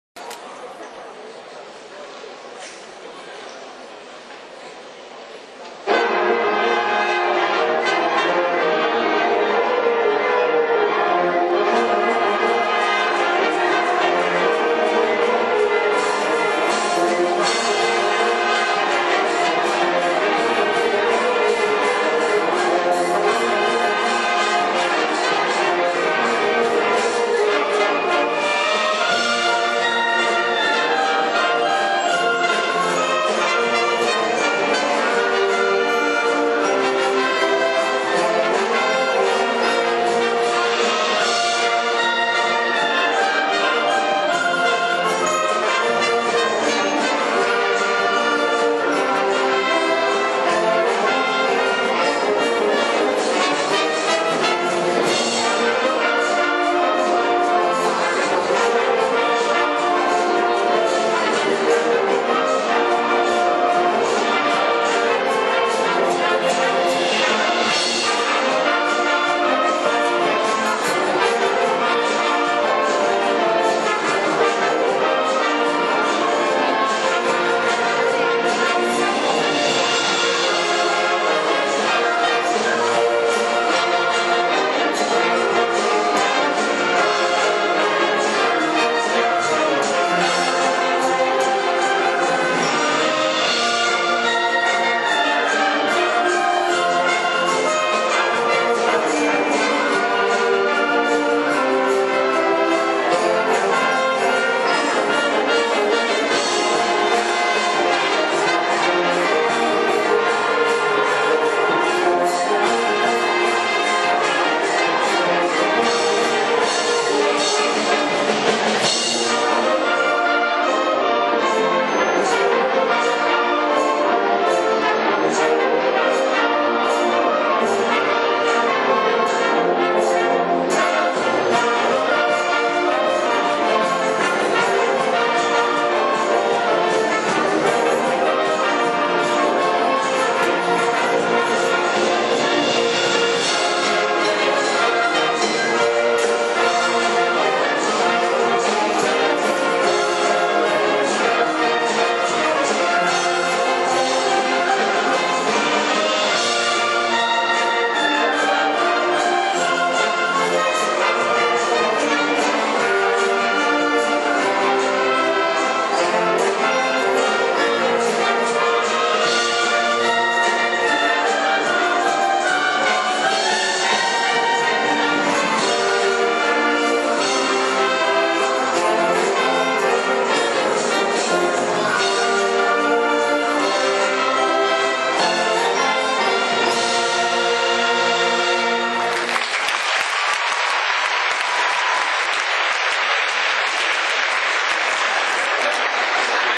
Foon Yew High School 90th School Anniversary Performance
Foon Yew Orchestra performed when 90th School Anniversary on 2003